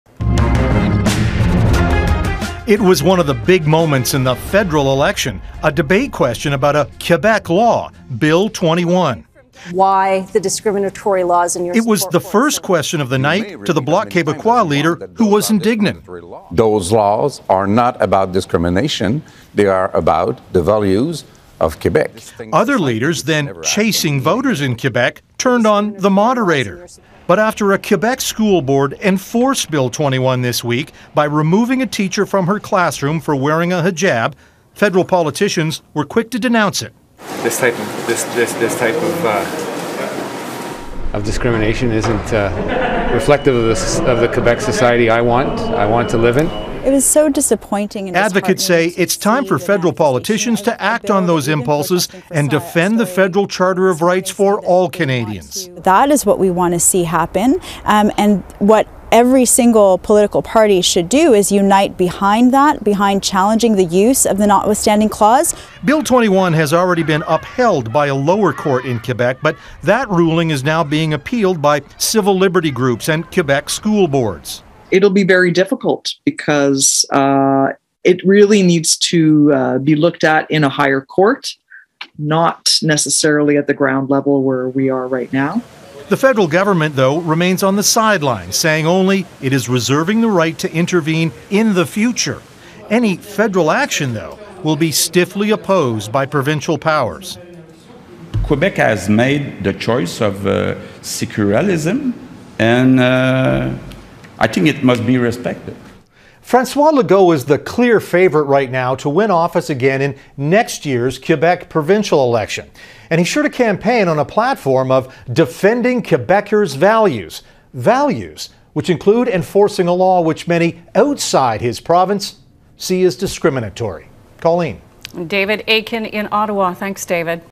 Bill 21: Feds stay on the sidelines as Quebec's controversial secularist law back in the spotlight
As controversy again erupts over Quebec's Bill 21, after a teacher wearing a hijab was removed from her classroom, calls are growing for the federal government to intervene in the religious symbols law.

In a statement sent to Global News on Friday, Prime Minister Justin Trudeau's Office said their position on the bill "has always been clear." "Nobody in Canada should ever lose their job because of what they wear or their religious beliefs," a spokesperson for the PMO said.

But as David Akin explains, despite the denouncements, Ottawa is choosing to stay on the sidelines.